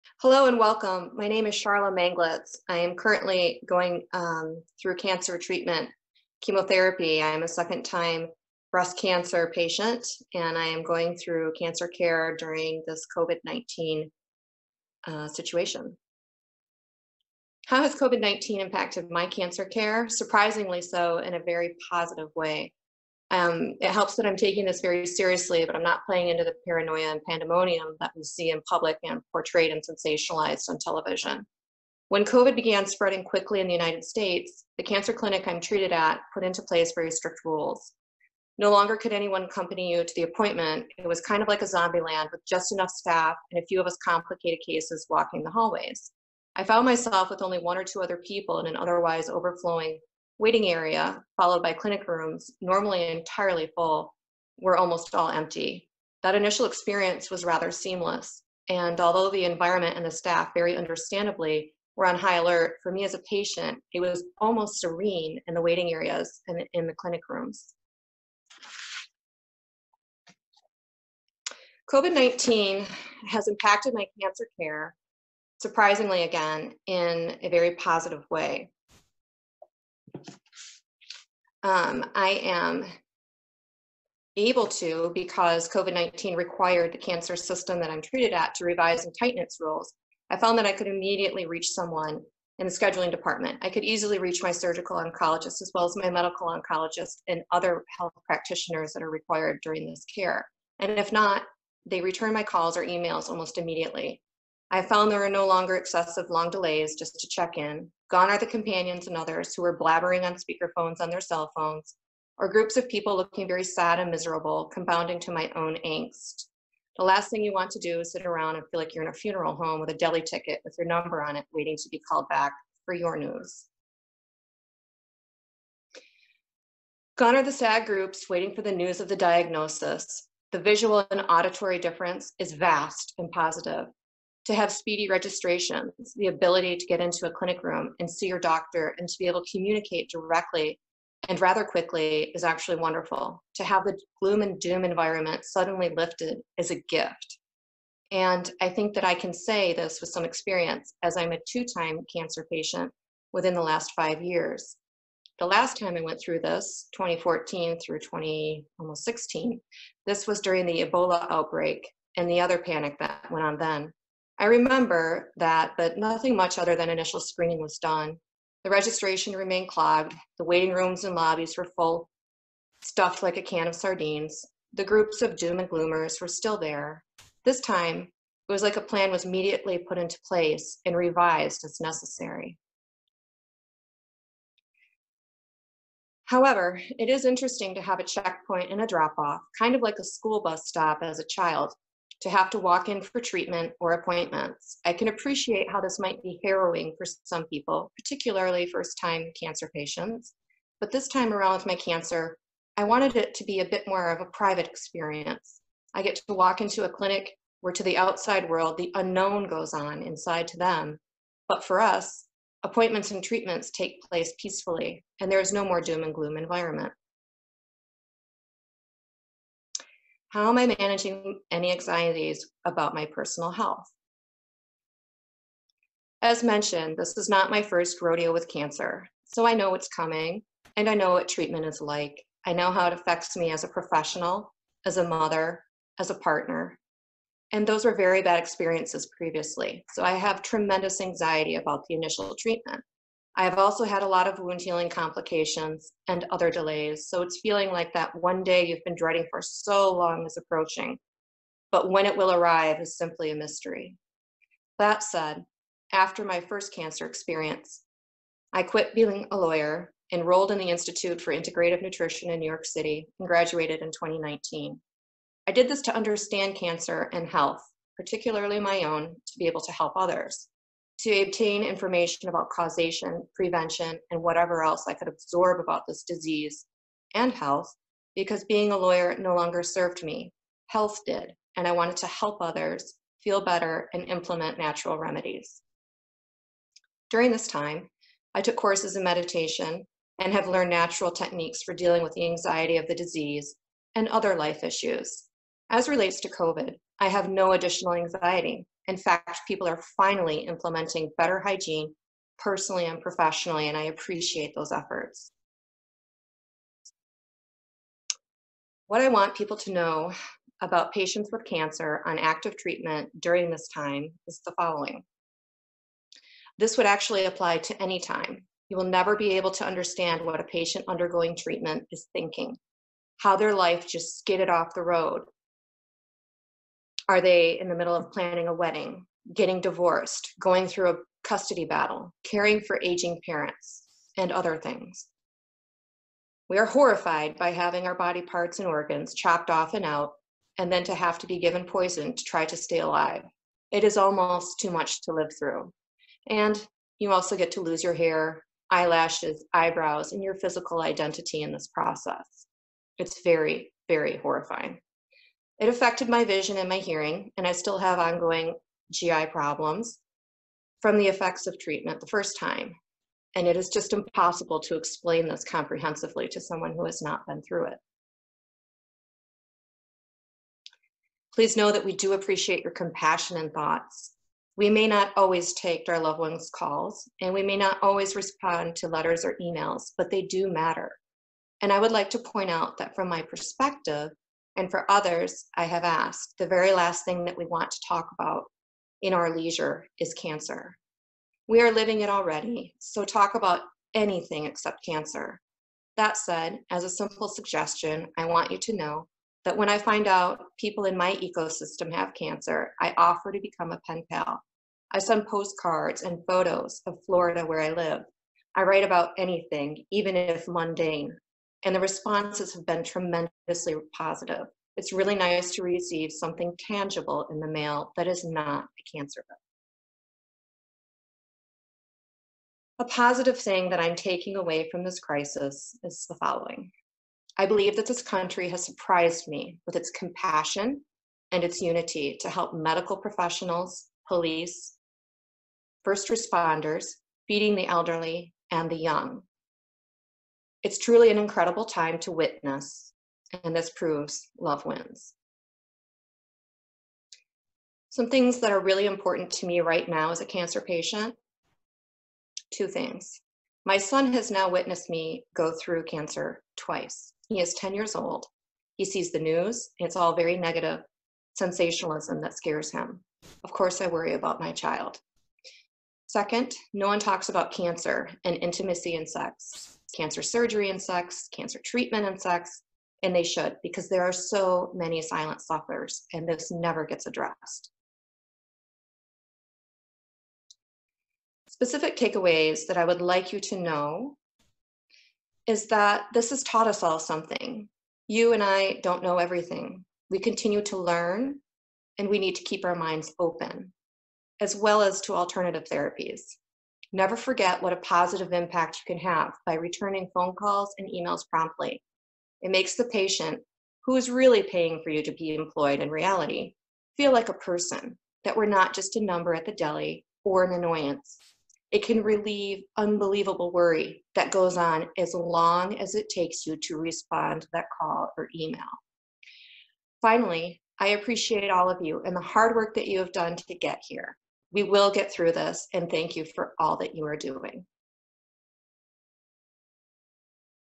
2020 Cancer Care in the Time of COVID-19 | Patient Perspective
From our 2020 Cancer Care in the Time of COVID-19 Conference, we bring you a session featuring a patient perspective by Sharla Manglitz.

To obtain CME/CE credits you must participate in the full 2020 Cancer Care in the Time of COVID-19Conference, click here to view all sessions

Information on obtaining CME/CE Credits can be found in the playlist description.